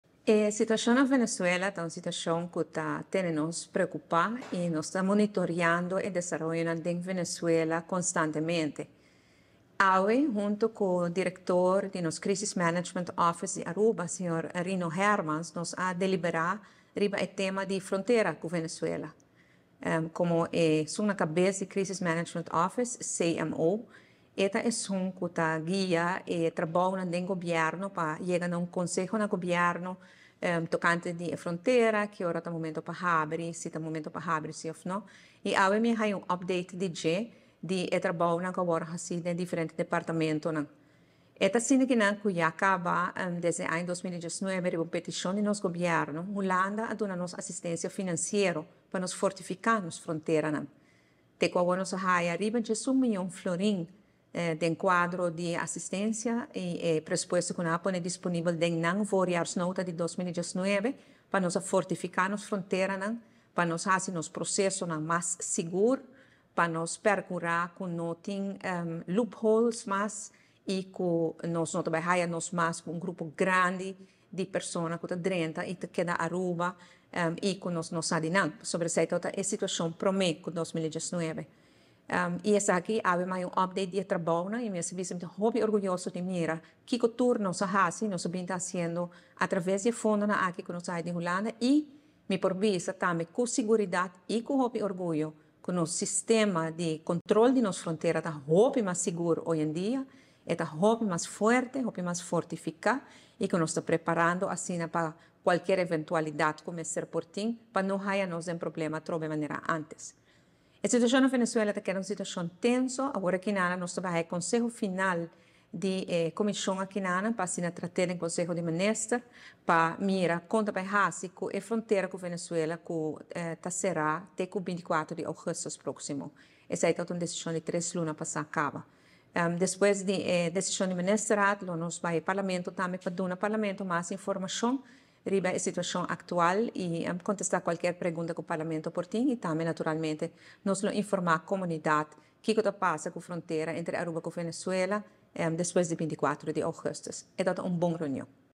Reunion CMO
Prome Minister Evelyn Wever-Croes a reuni cu hefe di Crisis Management Office, Rino Hermans, pa ricibi un update riba e tema di e frontera cu Venezuela. A discuti extensamente riba e situacion na Venezuela cu ta un situacion cu tin e preocupacion y atencion di Gobierno di Aruba y Gobierno constantemente ta monitorea e desaroyonan na e pais bisiña.